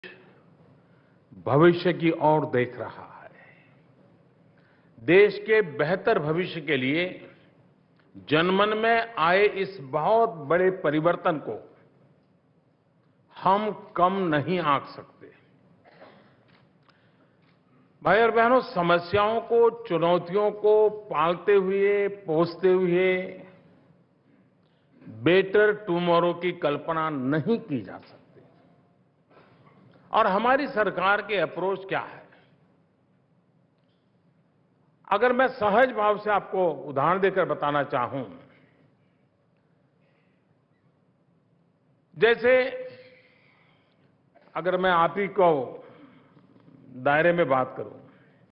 HT Leadership Summit में PM Modi ने Bhimrao Ambedkar को दी श्रन्धांजलि
PM Modi का संबोधन HT Leadership Summit में, जानिए लोगों से क्या कहा | पीएम मोदी ने दी Dr Ambedkar को श्रन्धांजलि